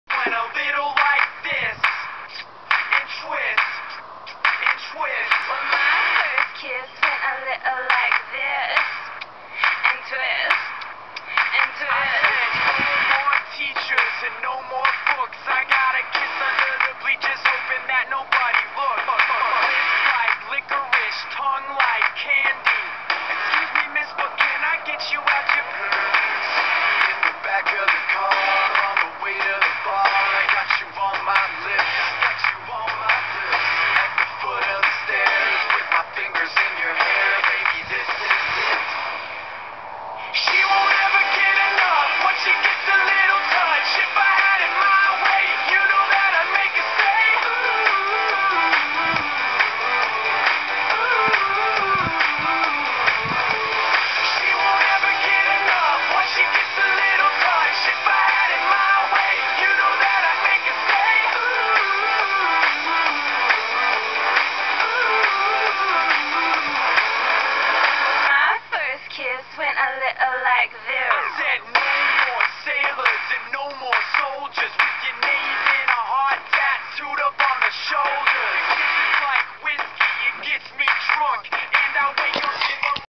Gymnastics
i wasbored and i was thinking of things to do so i thought hey, wh dont i just make a video of me doing some gymnastics. But then i remembered i had a hurt knee. But still i made a video of all the stuff that were easy:) and thats why my bhs is so ugly cuz of my knee